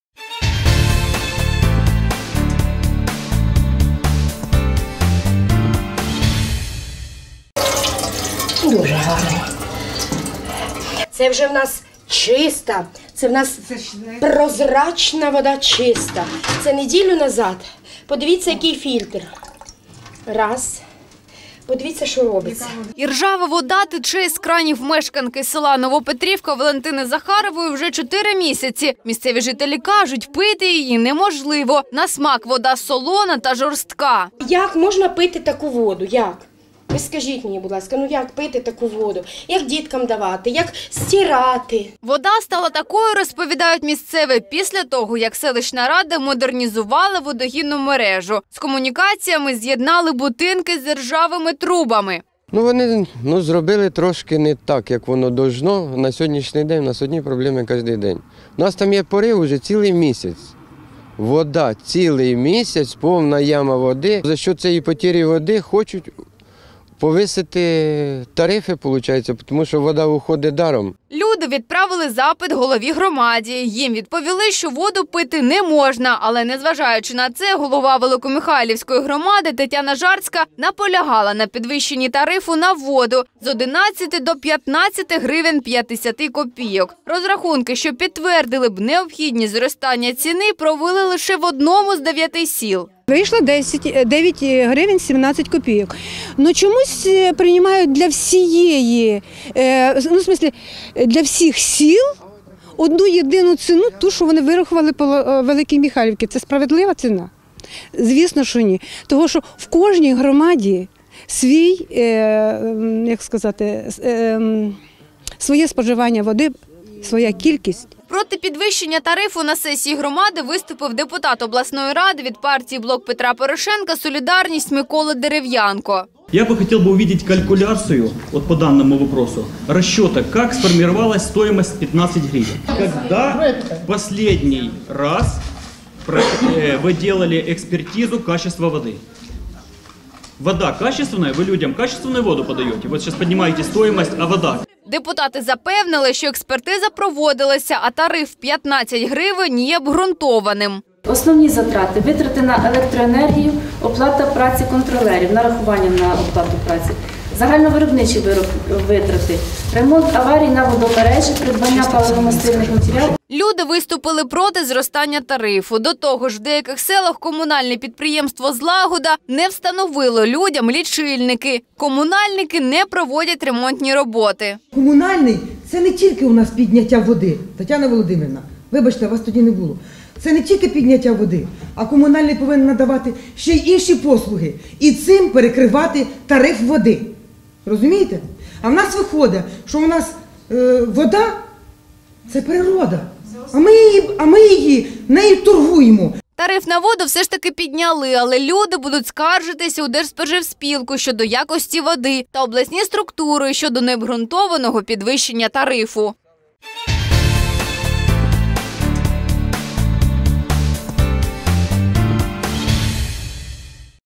У Великомихайлійській громаді підвищили тариф на воду
Іржава вода тече із кранів мешканки села Новопетрівка Валентини Захарової вже 4 місяці. Місцеві жителі кажуть –пити її неможливо. На смак вода солона та жорстка.